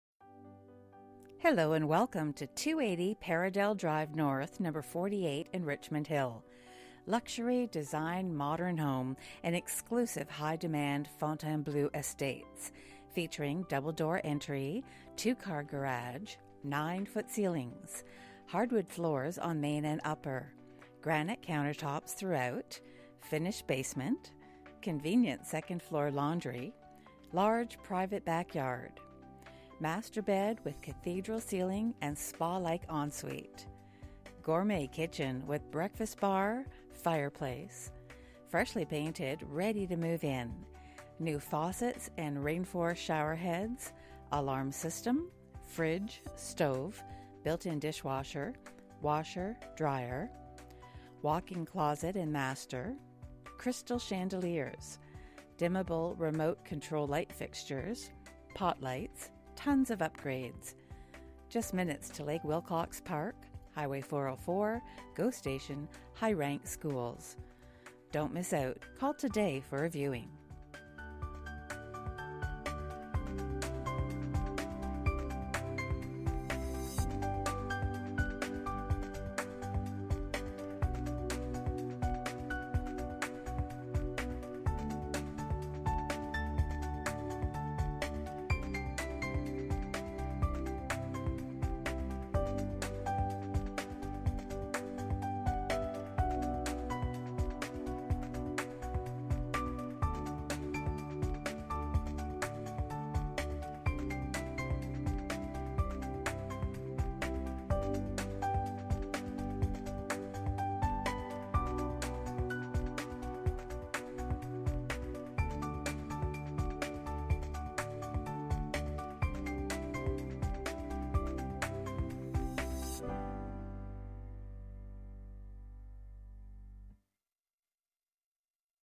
280 paradelle Drive N #48, Richmond Hill
Luxury design Modern Home in exclusive high demand Fontainbleau estates. Featuring Double door entry,2 car garage, 9 ft Ceilings,
Hardwood floors on Main&Upper, Granite countertops throughout, Finished basement, Convenient 2nd floor Laundry, Large private backyard, Master bed
w/Cathedral Ceiling & Spa Like En-Suite. Gourmet Kitchen with breakfast Bar, Fireplace,  Freshly painted ready to move in, New faucets and rainforest shower heads, Alarm system, fridge, stove, B/I dishwasher, washer, Dryer, walking
closet in master, Crystal chandeliers, dimmable remote control light fixtures, pot lights ,Tons of upgrades.. minutes to Lake Wilcox park, Hwy 404, Go
Station, High Ranked Schools.

Ariel Smilovici
Century 21 Heritage Group Ltd.
17035 Yonge St Suite 100
Newmarket, L3Y 5Y1